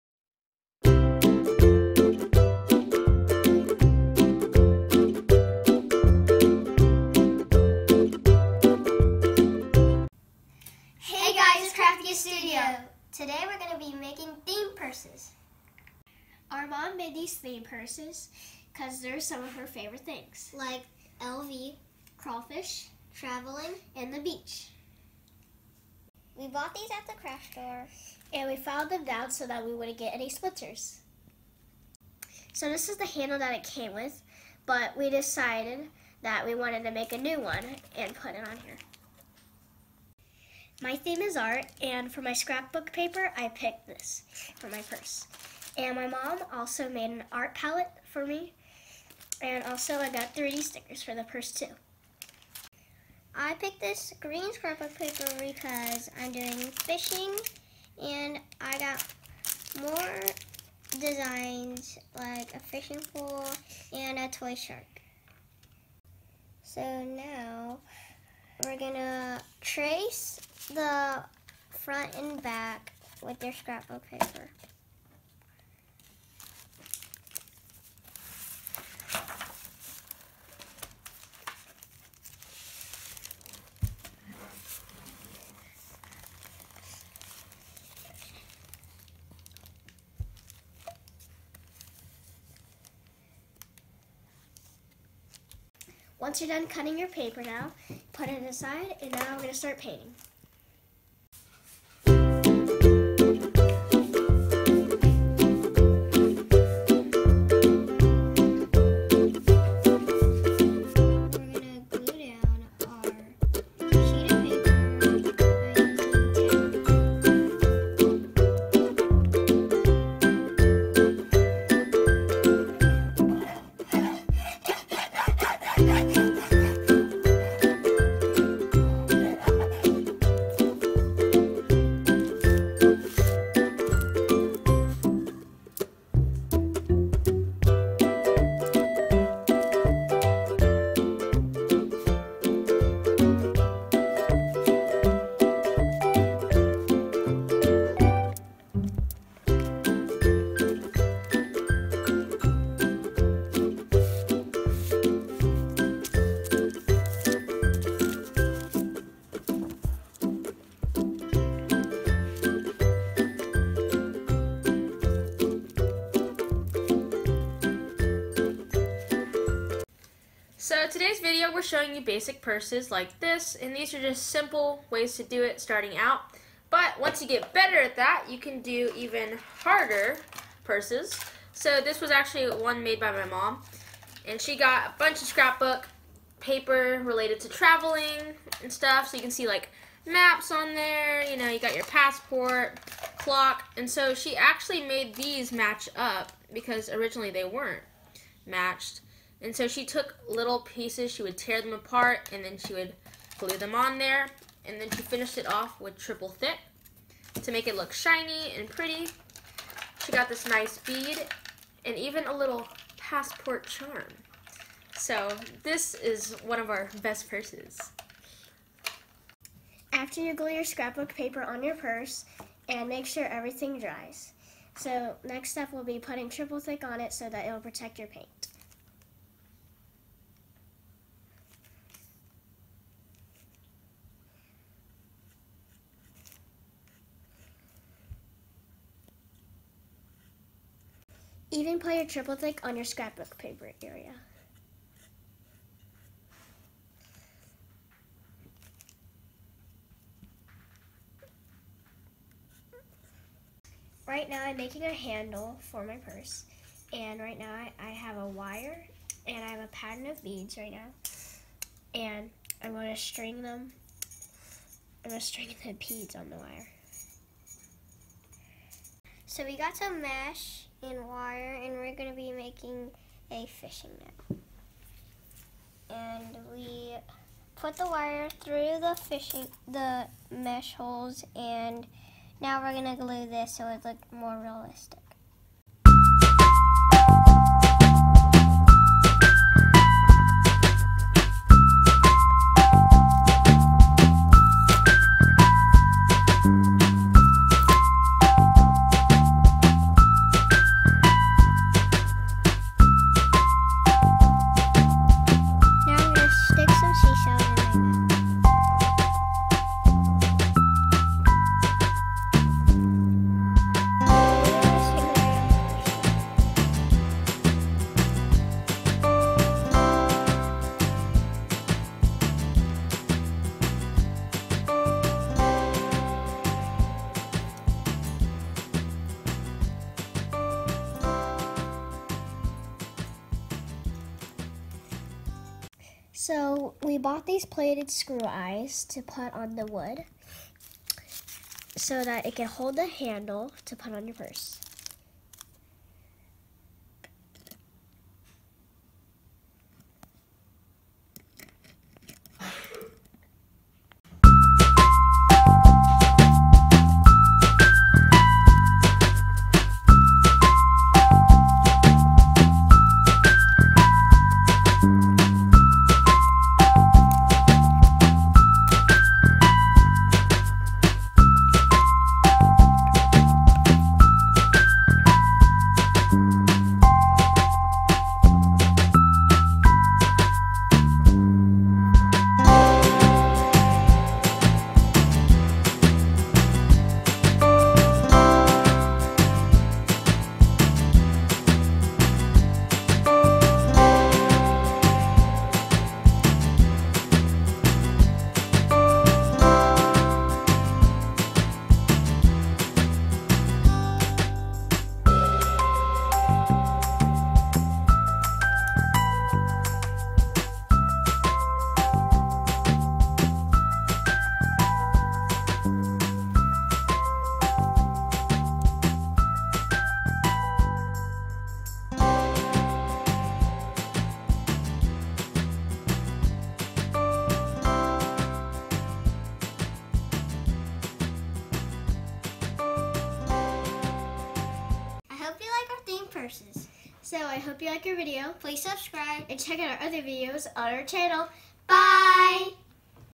Theme Purses
This is a Unique project making these boxy theme purses. CraftyKids will teach you how to make a basic, beginners level personalized handbag!!